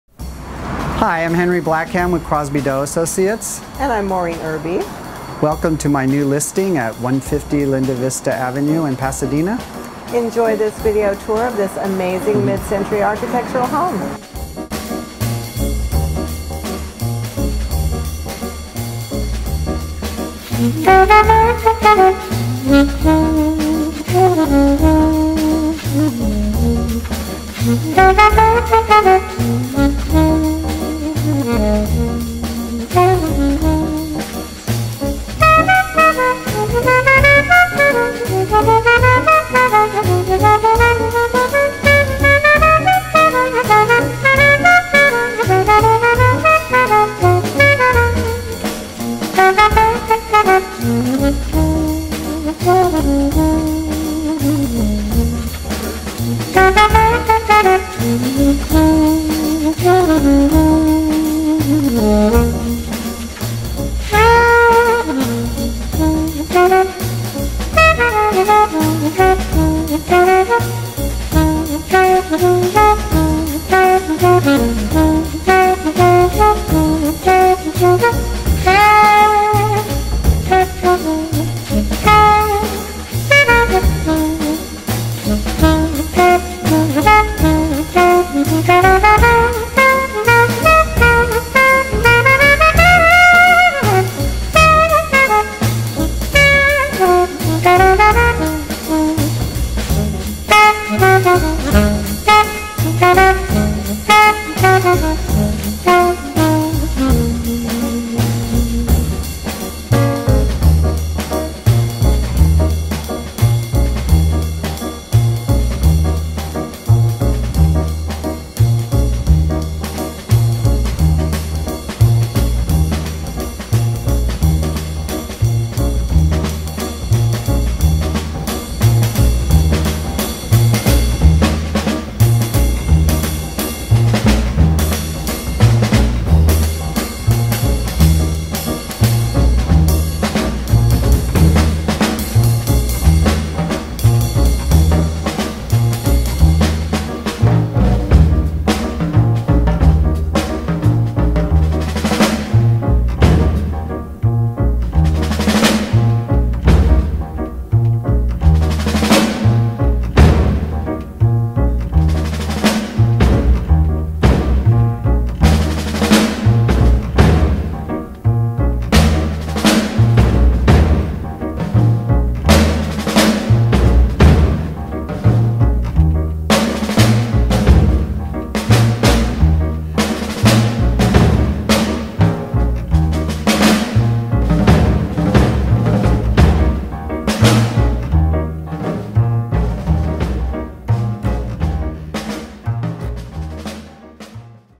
John Lawrence Pugsley AIA :: 150 Linda Vista Ave, Pasadena
Designed in 1959 by John Lawrence Pugsley AIA, this classic mid century is located in Pasadena's coveted Linda Vista neighborhood. Perched above the Arroyo with expansive views, this architectural gem features walls of glass, a koi pond, a Japanese garden with outdoor shower, a dramatic atrium, and stunning stone work. Combining iconic mid century details with modern amenities, the 2401 sq ft indoor / outdoor home features 3 bedrooms/3 baths with designer fixtures, a cook's kitchen with European appliances, breakfast room, formal dining room, media room, wood floors, central air and heat, and recessed lighting indoors and out. An entertainer's delight, this indoor/outdoor home has an expansive 1300 sq ft multi-level deck, built-in BBQ and spa, indoor /outdoor sound system, outdoor fireplace, lush landscaping with mature oaks, and an automatic lighting and sprinkler system. A mid century oasis nestled in the trees, yet close to Pasadena's Old Town, Downtown LA, and Burbank studios.

150 Linda Vista Avenue,
Pasadena

Exclusively Represented by
Henry Blackham & Maureen Erbe